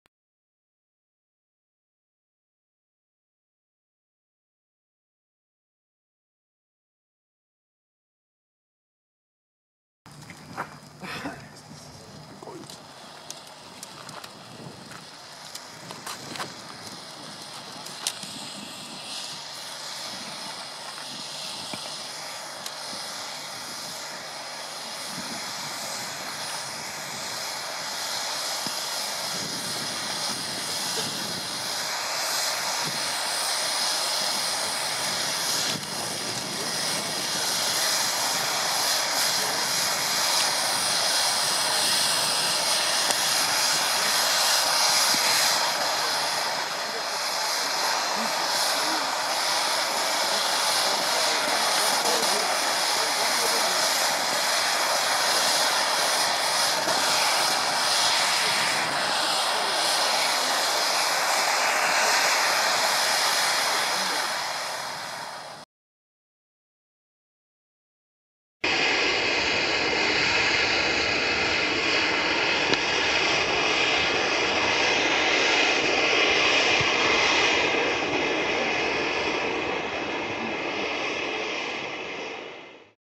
Blackboxx Mega Vulkan Feuersäule Gold | Kat 3 Vulkan
Aufgenommen beim Neumann Feuerwerk Vorschießen.
► Kommentieren